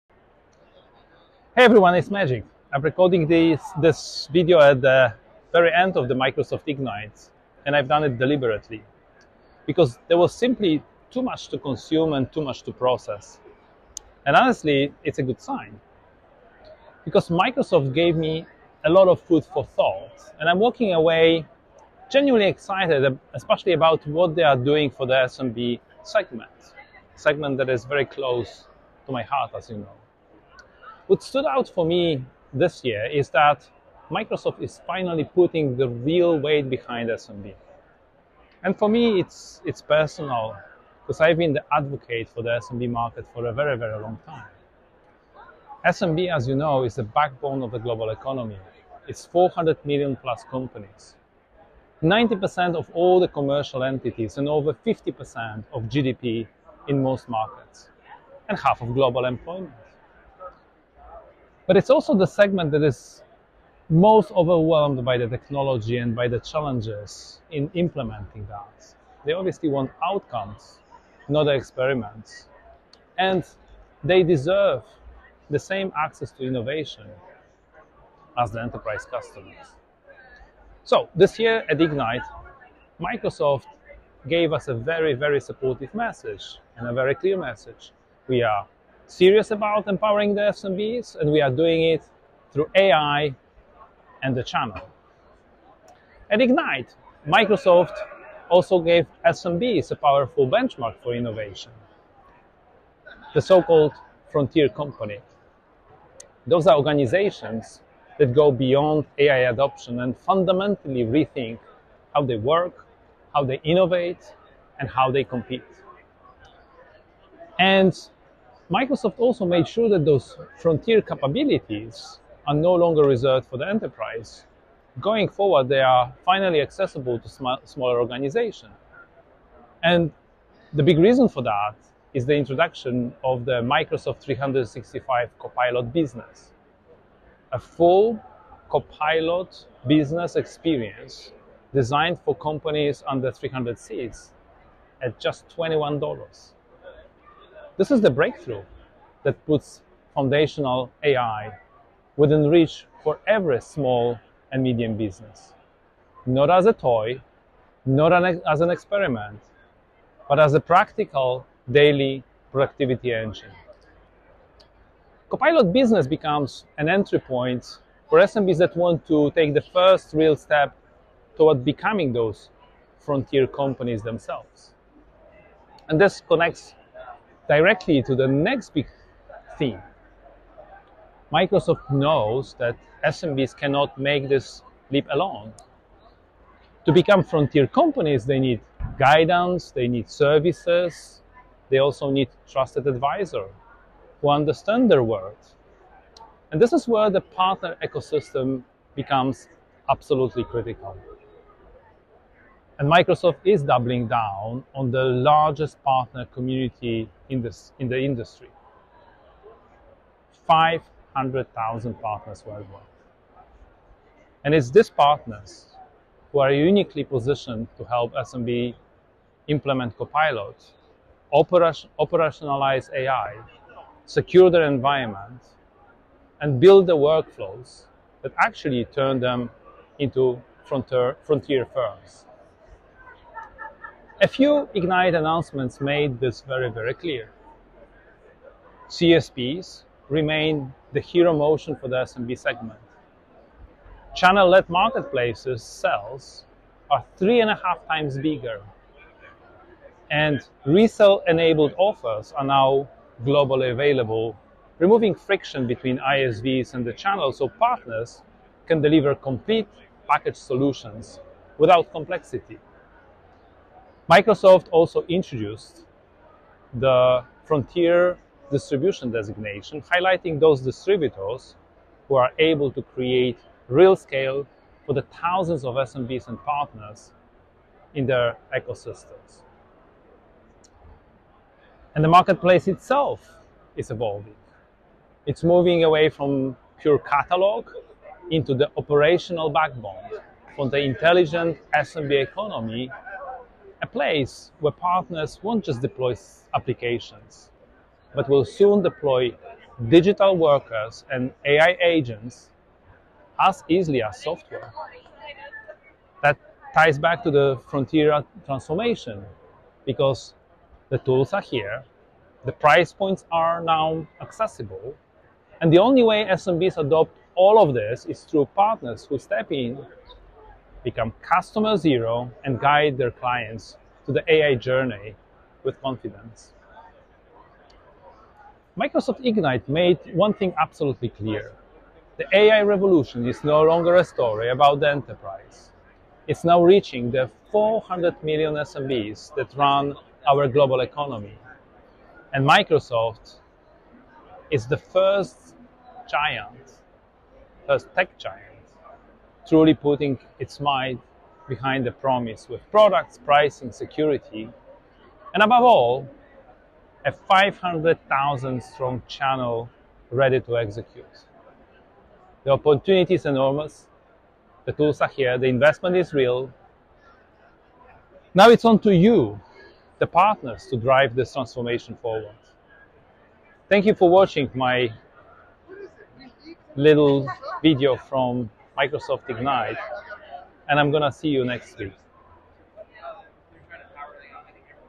AI for Every SMB! — My Quick Takeaways from Microsoft Ignite
I waited until the end of Microsoft Ignite to record my recap — there was simply too much to absorb.

But one thing is clear: SMBs are finally at the center of the AI conversation.
Microsoft defined what a Frontier company looks like and showed how even small businesses can get there — with tools like Copilot Business at $21 and a channel ecosystem ready to scale it.

I pulled my key takeaways into a short video for everyone working with SMBs, MSPs, or thinking about their own AI transformation.